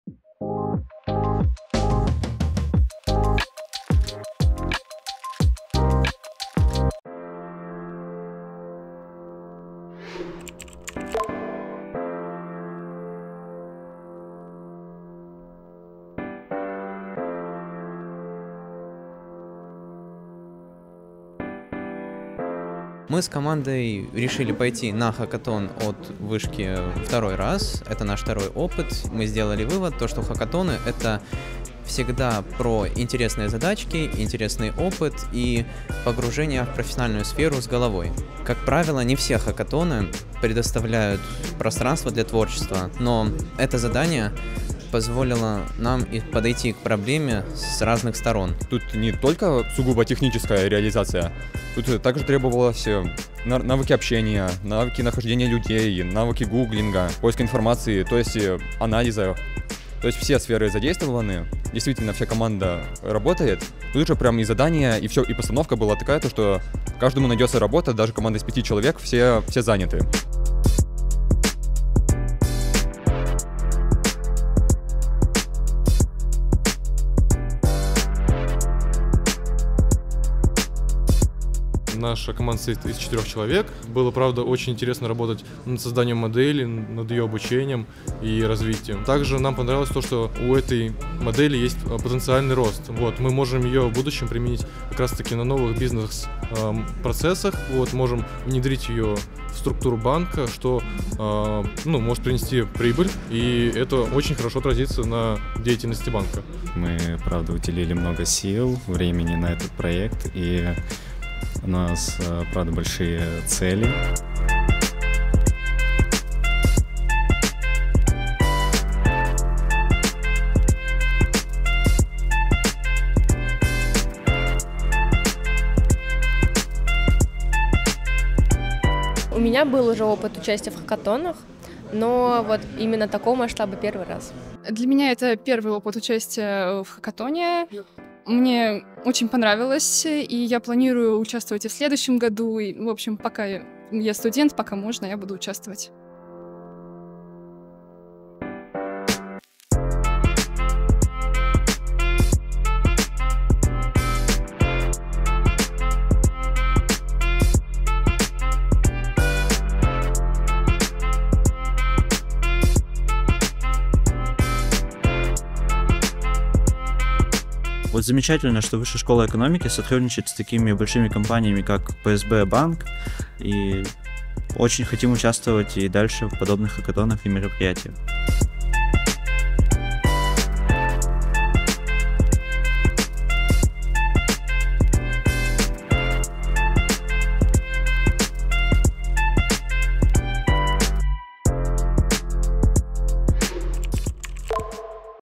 Хакатон ВШЭ ПСБ.Хак
Вышка Онлайн и ПСБ провели самый большой в НИУ ВШЭ хакатон по искусственному интеллекту для студентов.

Заключительный этап хакатона прошел с живым нетворкингом в кампусе университета. А мы пообщались с участниками и узнали их впечатления.